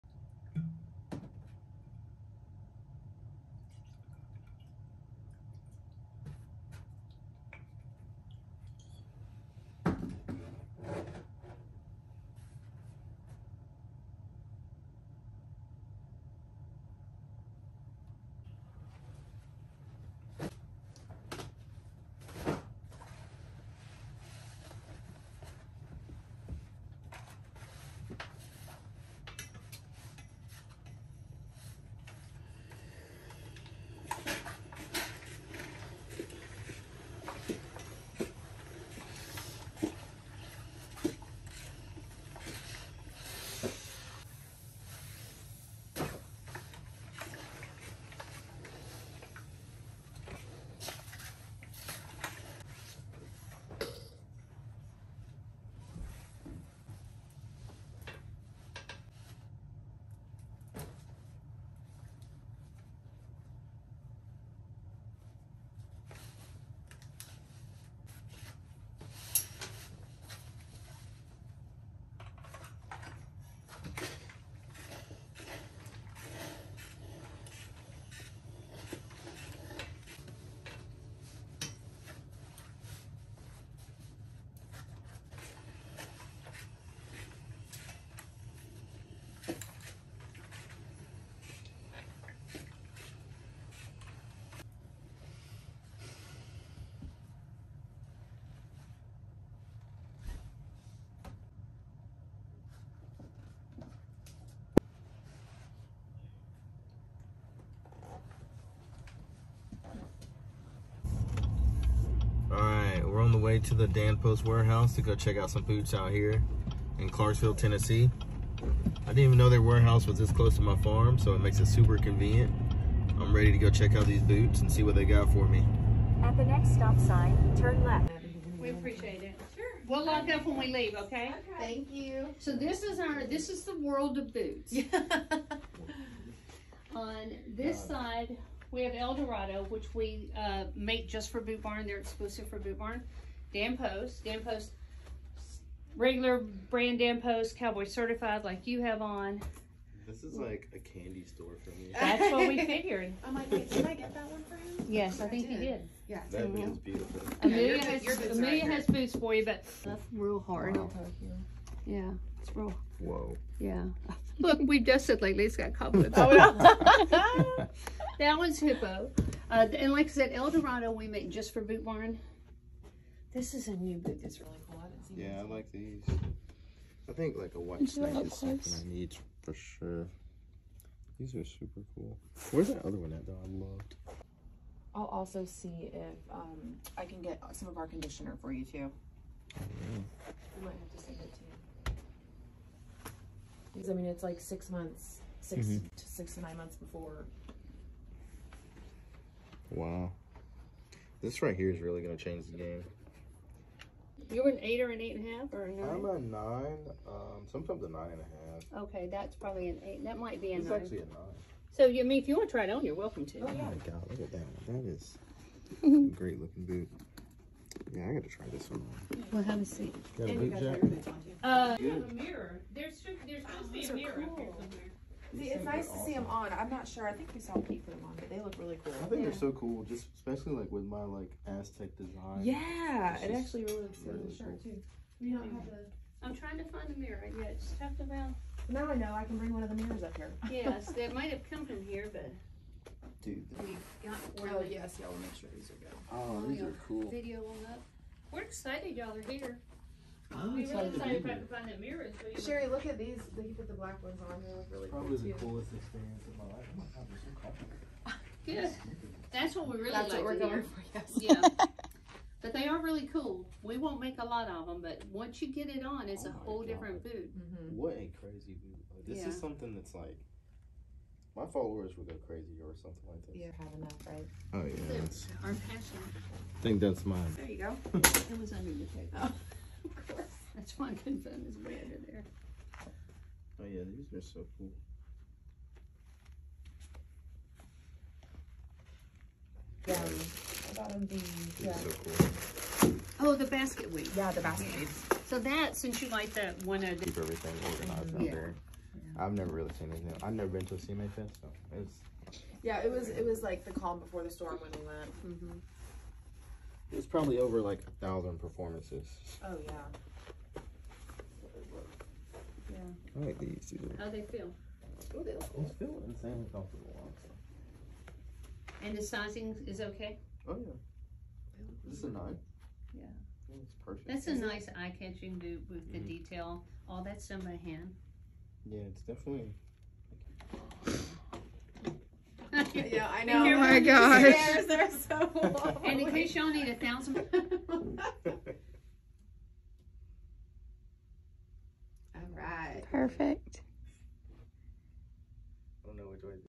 Going to check out new Cowboy Boots
Going to check out top secret cowboy boots with my new sponsor Dan Post